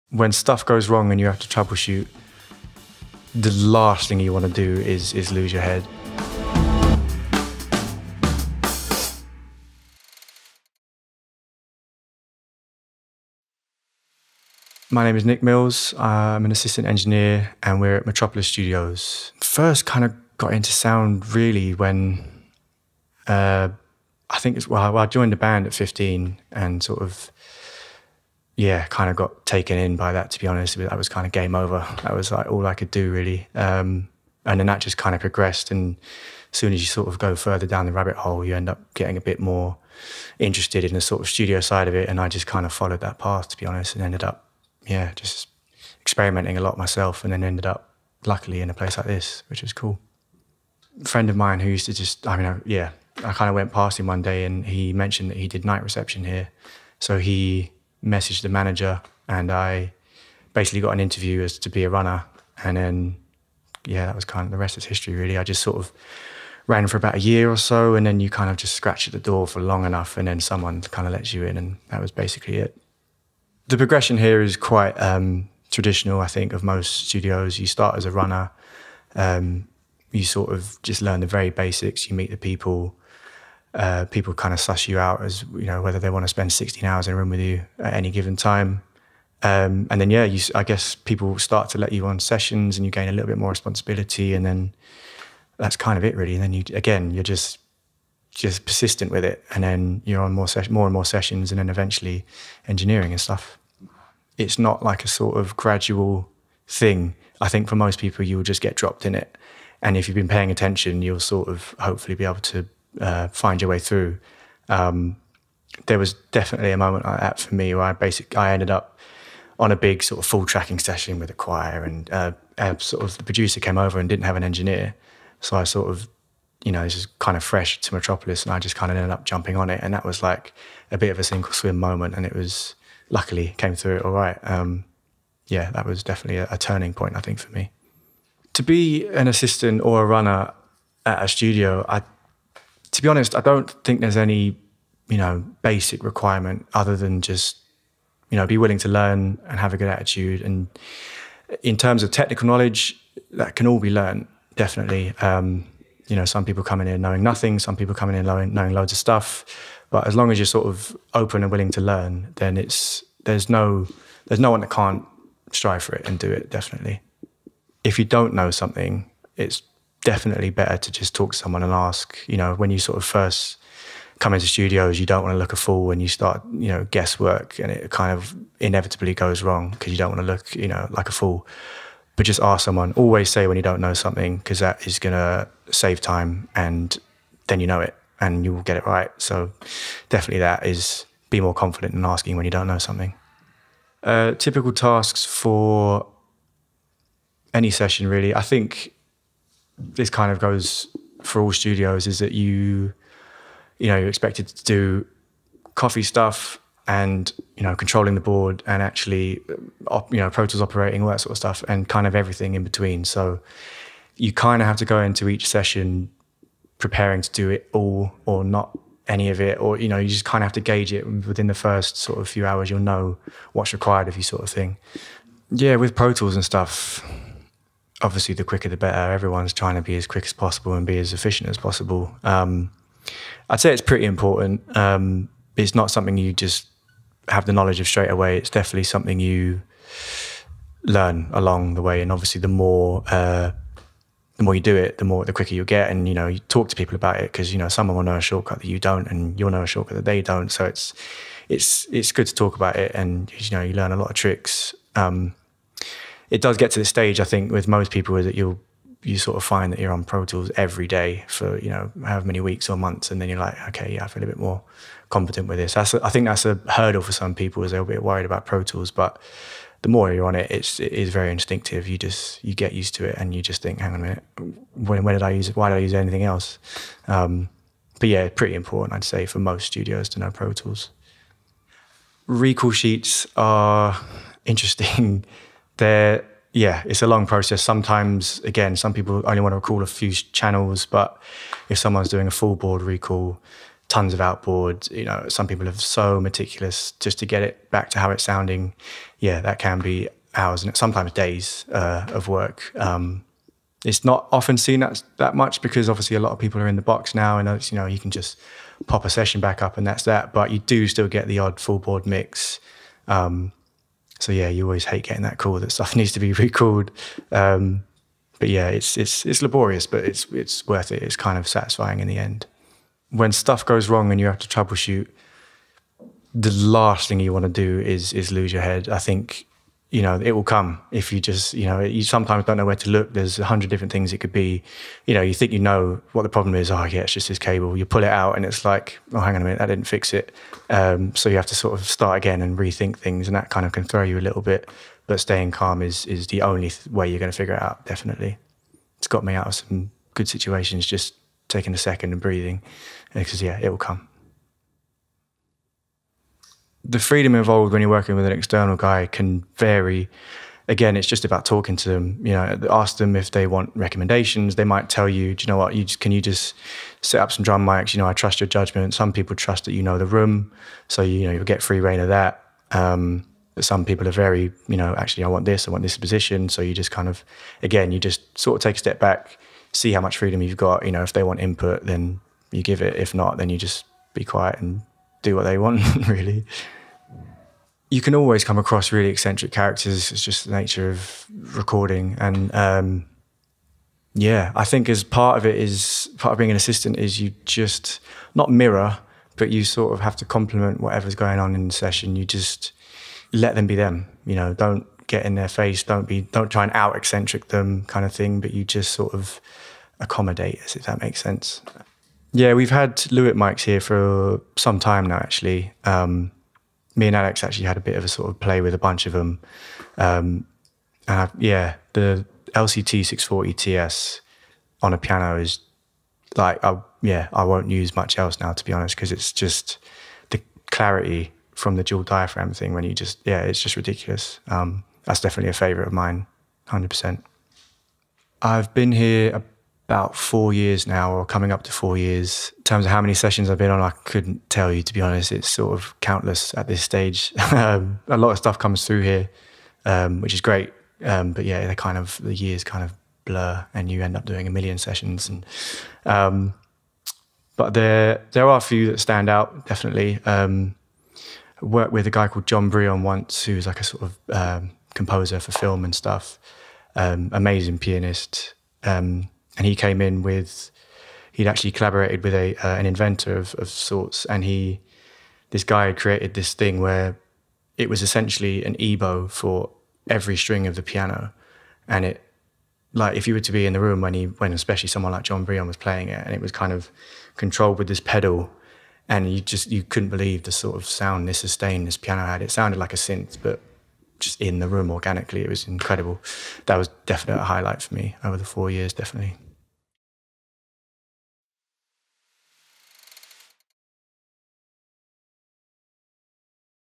How to be an assistant studio audio engineer
How do you become an audio engineer? For most people, it will start with an assistant role.
We sat down and talked with Nick Mills, assistant audio engineer at the world-famous Metropolis Studios in London.
He talks about his duties in the studio, how he got his chance to go from a runner to an assistant audio engineer, what skills are essential, and how to climb the recording studio career ladder.